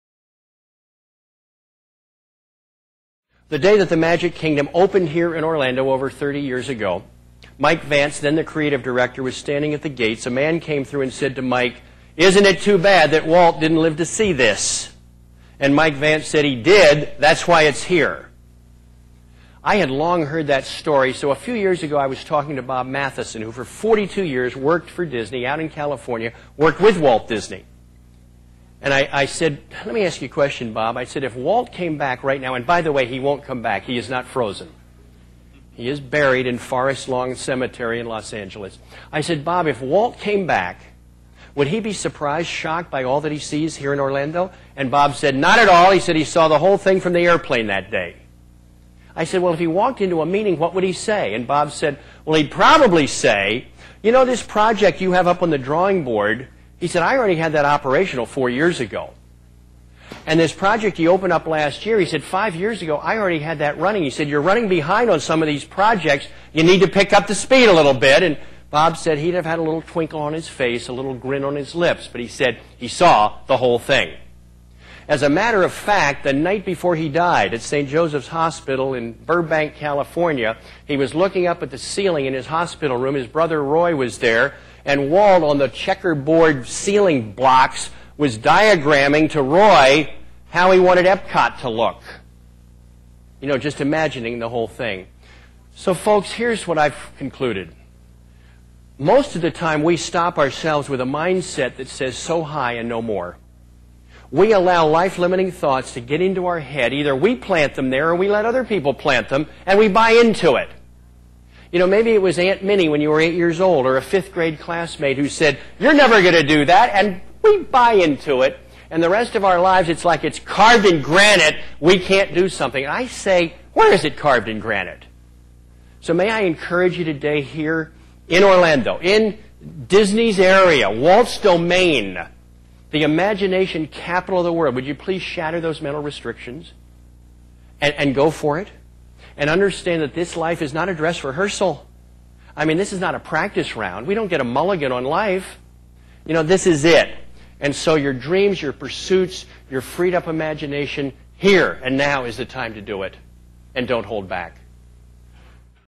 Pat Williams, Senior Vice President - Walt Disney's Five Secrets To Success -
Pat Williams is the co-founder and senior vice president of the NBA's Orlando Magic, professional speaker and author of over 75 books. Have Pat speak at your next event.